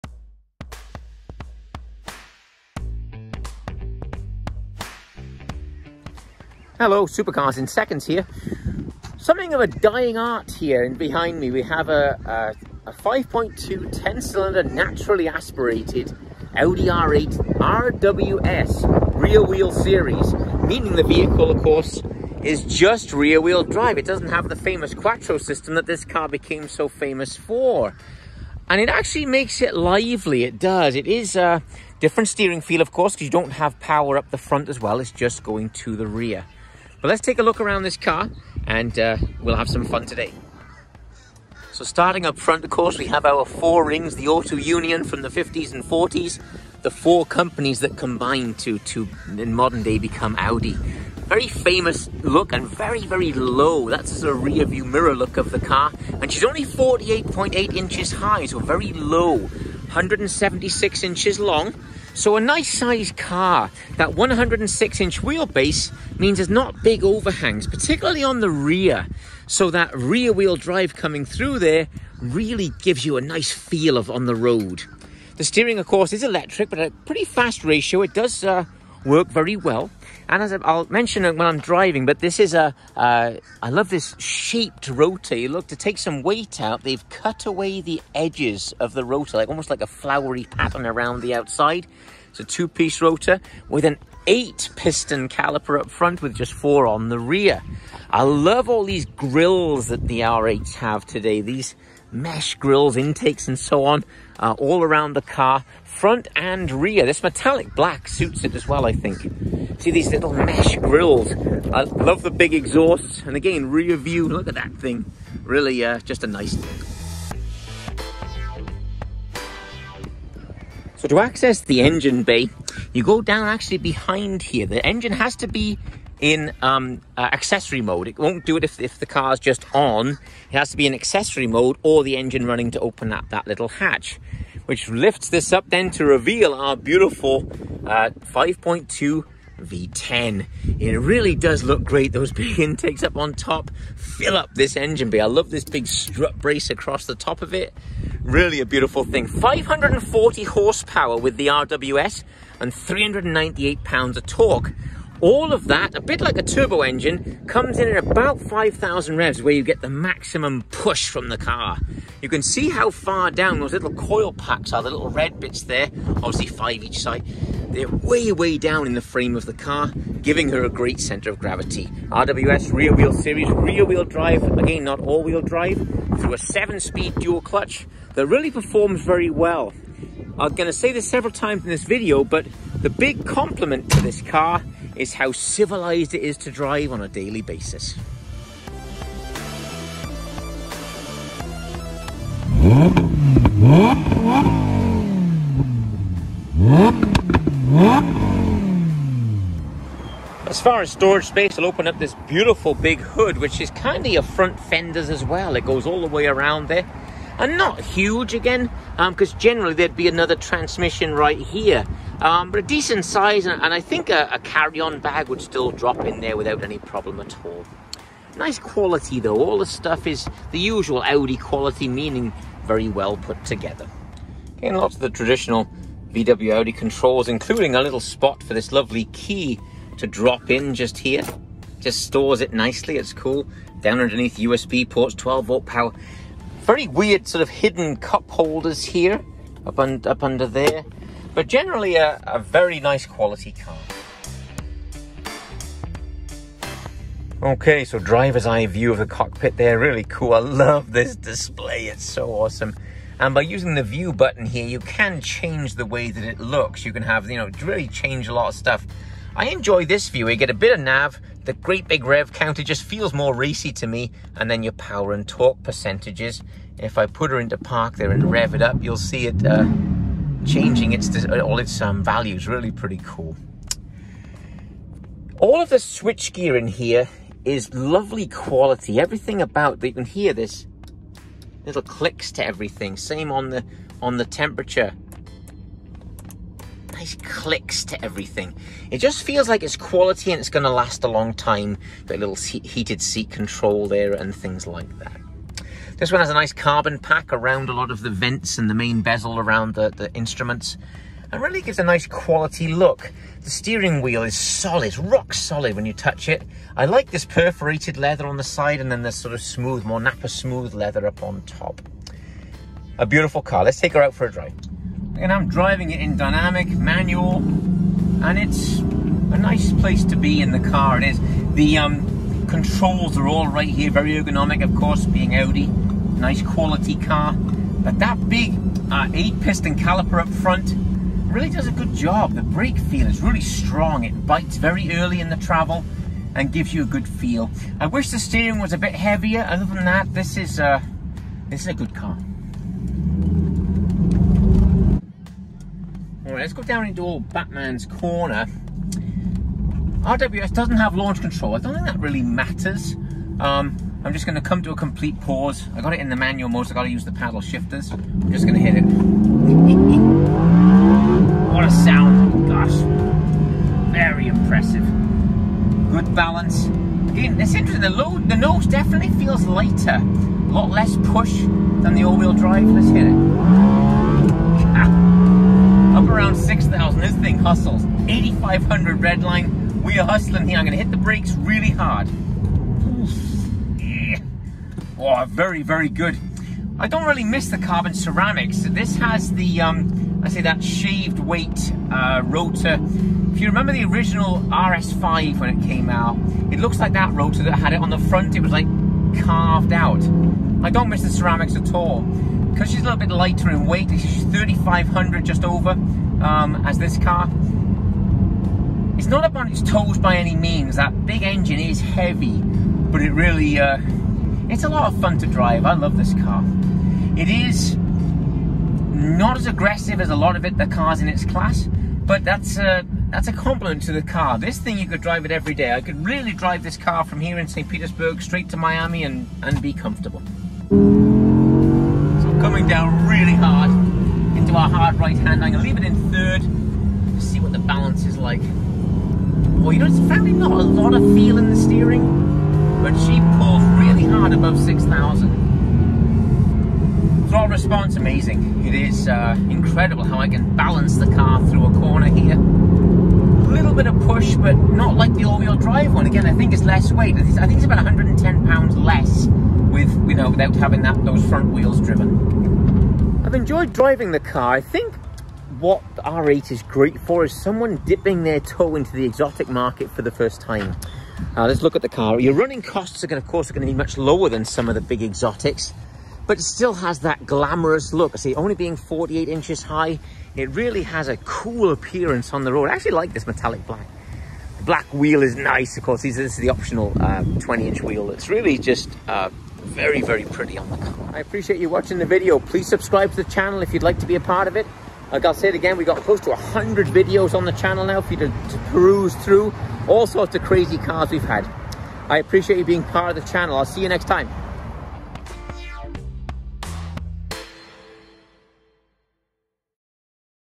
Audi R8 V10 RWS Review, exhaust and test drive .
Today we drive the Audi R8 V 10 RWS one of just 999 cars and push it to the limit